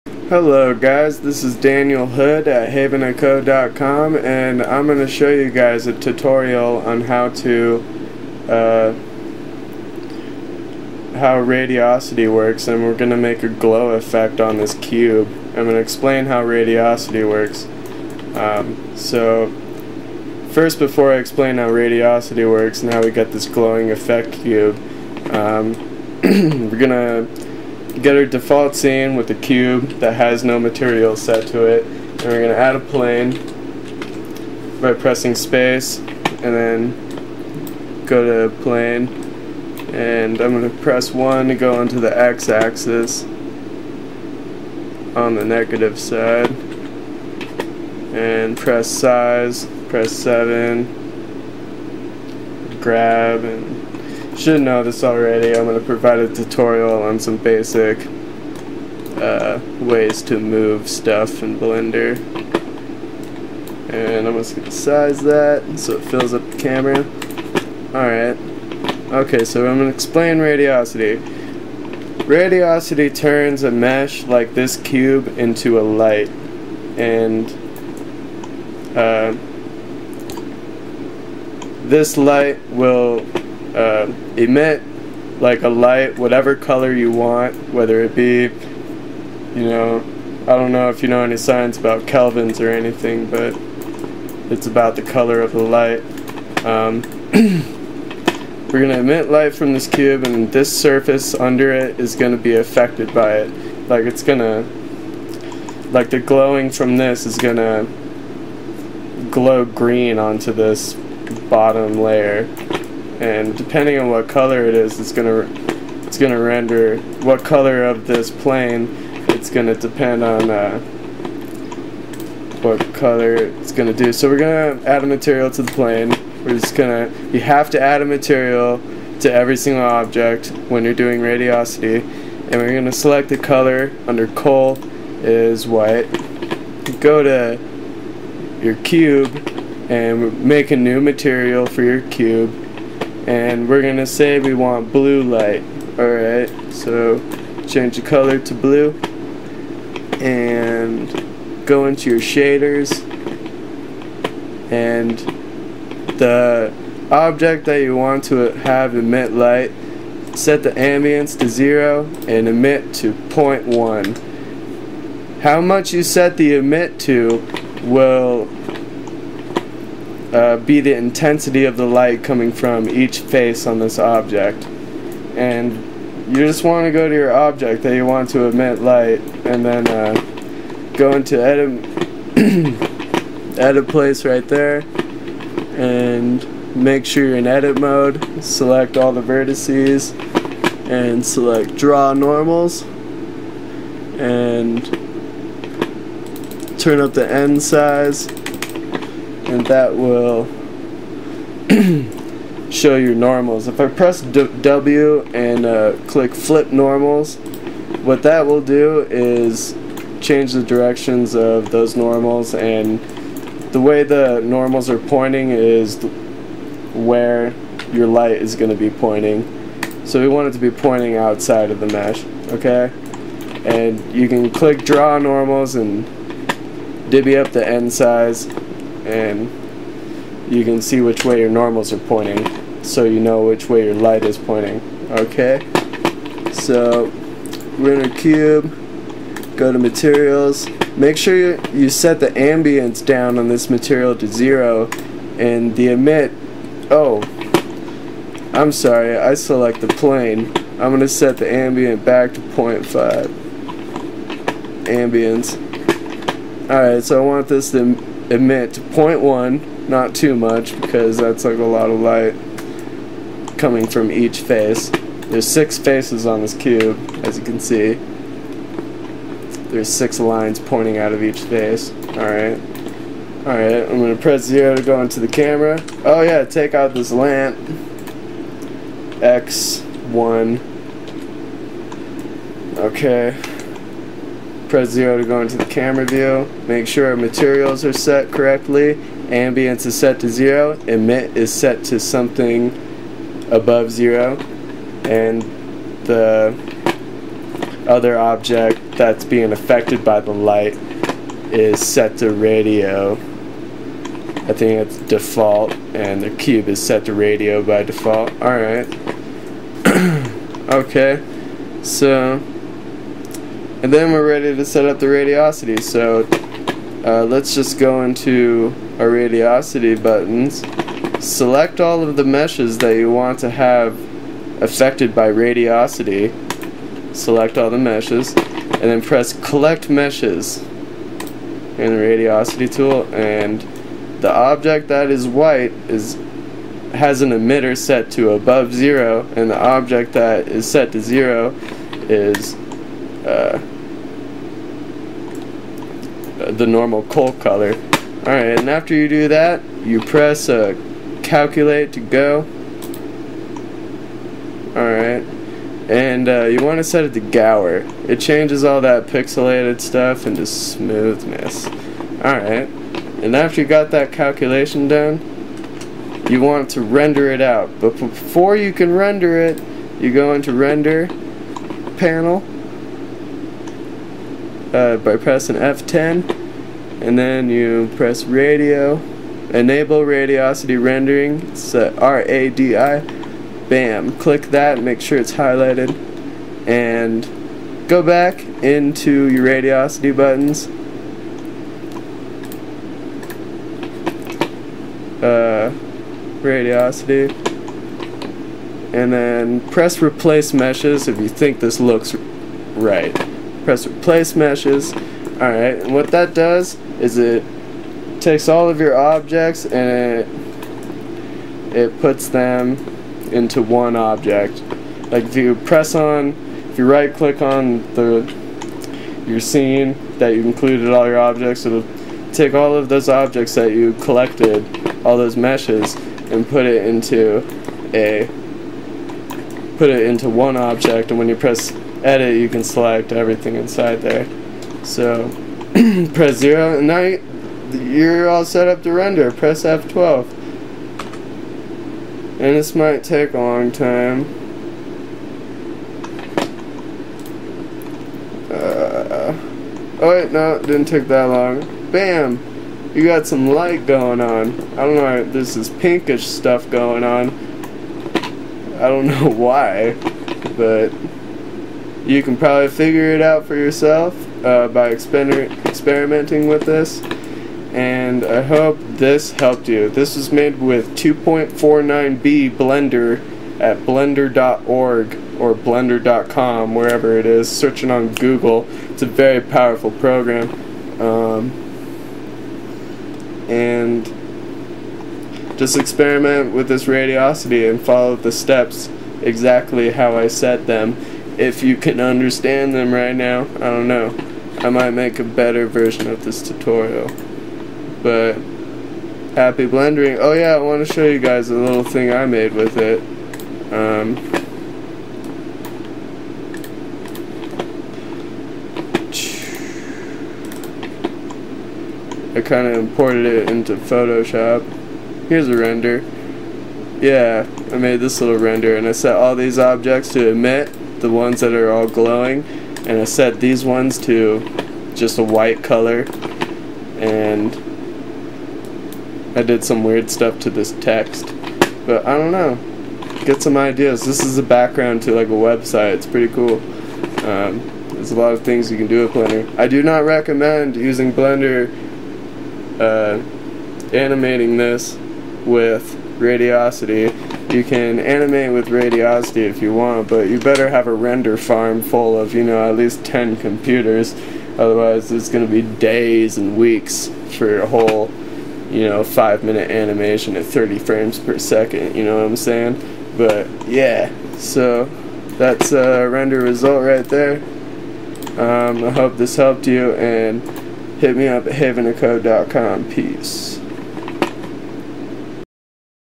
Blender Radiosity 2.49b Make Objects Glow - The Easy Tutorials
Its really easy to get Radiosity working in blender. Here are the steps.

1. Make plane and a cube on the scene. Delete any lamps or lights.
2. Make the plane a material, and set col to white color.
3. Make the Cube material with blue color in col. Also go to shaders, and make the ambience 0 and the emit to 0.1.
4. go to render panel, and make sure radio is highlighted to render radiosity.
5. select all the meshes (plane and cube, not camera), and then go to materials button then click on radiosity button.
6. press collect all meshes, then press "go" under calculate.
7. after calculation is done. press replace meshes,
8.  Then press F12 to render!